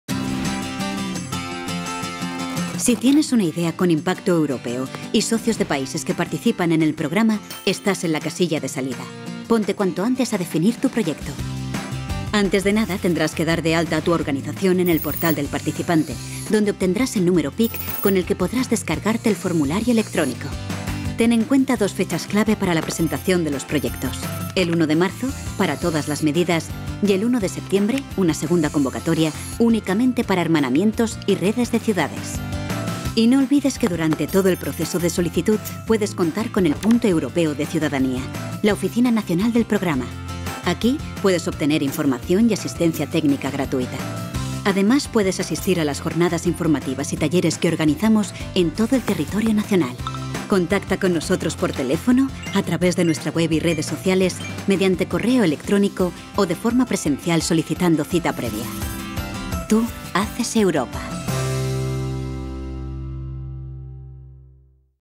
PROGRAMA EUROPA CON LOS CIUDADANOS.
Vídeo promocional del Programa Europa con los Ciudadanos y la oficina nacional del Programa, Punto Europeo de Ciudadanía.
Europa con los ciudadanos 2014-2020 pretende impulsar la participación activa de los ciudadanos de la Unión mediante la financiación de planes y actividades que promuevan el conocimiento de la historia y los valores compartidos de Europa, y que fomenten un sentido de responsabilidad a la hora de facilitar las condiciones de participación democrática en el funcionamiento futuro de la UE.
El PEC de España facilita estos servicios -totalmente gratuitos- en persona, telefónicamente y a través de esta página web, donde se ofrece documentación (convocatorias, formularios) e información relevante para los organismos y entidades interesados en el Programa.